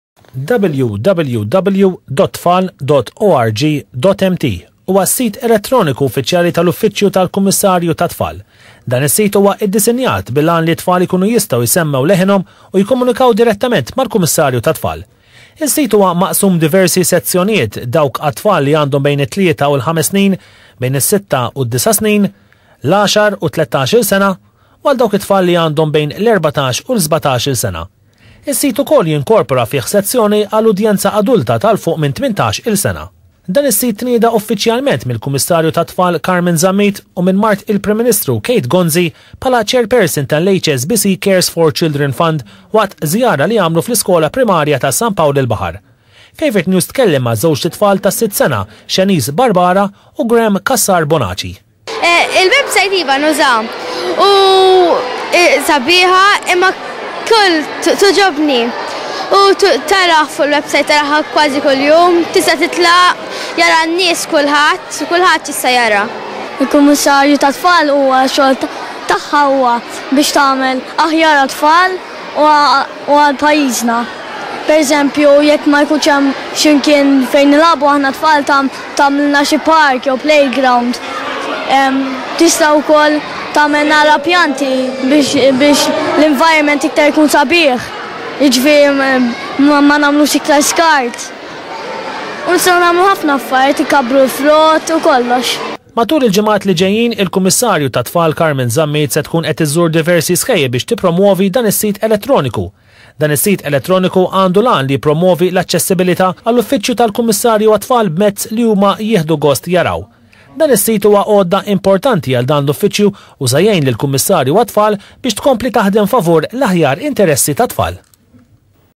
Launch of Commissioner for Children's Website at St Paul's Bay Primary (Favourite News)
Commissioner for Children Ms. Carmen Zammit and Chairperson of HSBC Cares for Children Fund Mrs. Kate Gonzi launched the new This event took place at our school during an activity held in the school hall. Guests were welcome by Head of School Ms. Lina Deguara and her Assistant Heads. During the activity children asked various questions to the Commissioner and Mrs. Gonzi and they also watched clips about how to make good use of the website. The mascot of the Commissioner for Children, Guginu, was also present for the activity and he showed a video of his recent song. The school choir also animated the activity. Mrs. Gonzi and Ms. Zammit also had the opportunity to visit classrooms in our school.